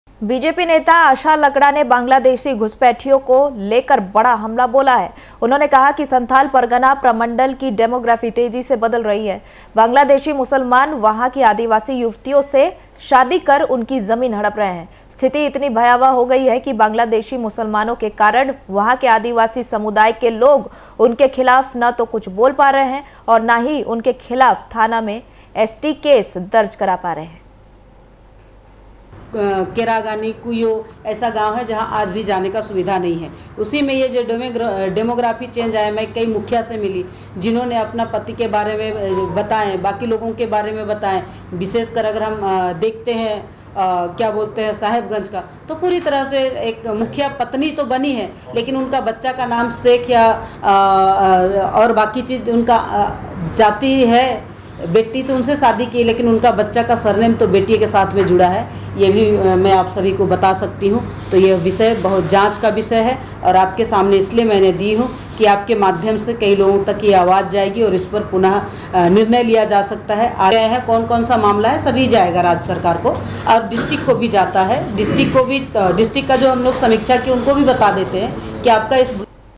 रांची : बांग्लादेशी घुसपैठियों पर बीजेपी आक्रामक, बीजेपी नेता आशा लाकड़ा ने साधा निशाना
Raftaar Media